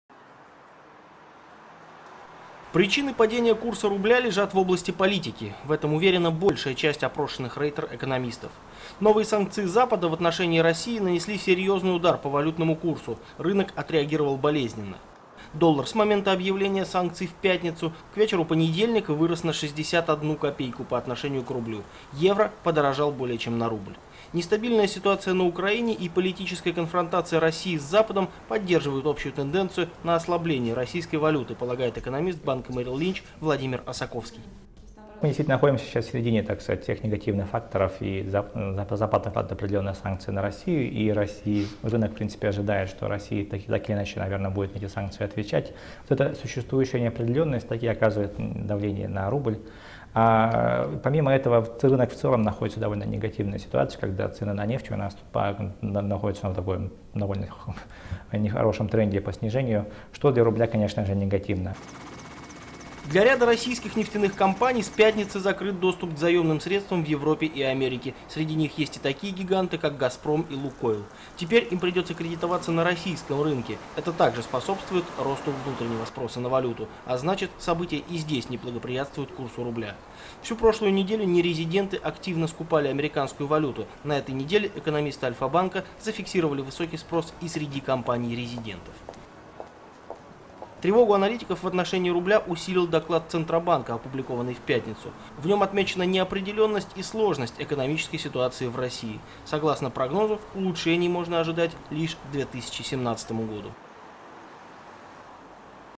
Курс рубля упал до исторического минимума (новости)
] 38 рублей за доллар. Таким курсом открылись валютные торги в понедельник утром. Рубль продолжает дешеветь. Прогнозы аналитиков по большей части неблагоприятны. Политическая ситуация негативно сказывается на стоимости российской валюты.

Причины падения курса рубля лежат в области политики — в этом уверена большая часть опрошенных Рейтер экономистов. Новые санкции Запада в отношении России нанесли серьезный удар по валютному курсу: рынок отреагировал болезненно. Доллар с момента объявления санкций в пятницу к вечеру понедельника вырос на 61 копейку по отношению к рублю, евро подорожал более чем на рубль. Нестабильная ситуация на Украине и политическая конфронтация России с Запада поддерживают общую тенденцию на ослабление российской валюты, полагает экономист банка Merril Lynch Владимир Осаковский.

ВЛАДИМИР ОСАКОВСКИЙ, ГЛАВНЫЙ ЭКОНОМИСТ ПО РОССИИ И СНГ BANK OF AMERICA MERRILL LYNCH:

«Мы находимся сейчас в середине негативных факторов. Запад накладывает санкции на Россию, и рынок ожидает, что Россия, так или иначе, наверное, будет на эти санкции отвечать. Существующая неопределенность оказывает давление на рубль. Помимо этого, рынок в целом находится в негативной ситуации, когда цены на нефть находятся в нехорошем тренде по снижению, что для рубля, конечно, негативно».

Для ряда российских нефтяных компаний с пятницы закрыт доступ к заемным средствам в Европе и Америке. Среди них есть и такие гиганты, как Газпром и Лукойл. Теперь им придется кредитоваться на российском рынке. Это также способствует росту внутреннего спроса на валюту, а значит, события и здесь не благоприятствуют курсу рубля. Всю прошлую неделю нерезиденты активно скупали американскую валюту, на этой неделе экономисты Альфа-Банка зафиксировали высокий спрос и среди компаний-резидентов.

Тревогу аналитиков в отношении рубля усилил доклад Центробанка, опубликованный в пятницу. В нем отмечена неопределенность и сложность экономической ситуации в России. Согласно прогнозу, улучшений можно ожидать лишь к 2017 году.

Источник ]- Книга, которая потрясла мир. Он уже никогда не будет прежним!